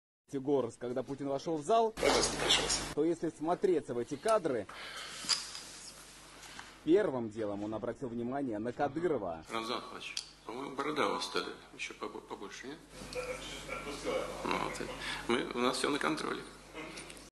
بوتين للرئيس الشيشاني: "لحيتك ازادت طولًا".. شاهد.. رد الأخير
وثق مقطع فيديو، موقف طريف، بين الرئيس الروسي فلاديمير بوتين، ونظيره الشيشاني رمضان قديروف.
وأظهر مقطع الفيديو، الرئيس بوتين وهو يمازح قديروف، بسبب طول لحيته، خلال جلسة عقدت بينهما.
وقال بوتين لـ قديروف: "رمضان أخماتوفيتش، لحيتك ازدادت طولا"، ليجيبه الأخير: "لقد أطلقتها".
ورد بوتين على قديروف قائلًا: "لا يفوتنا شيء".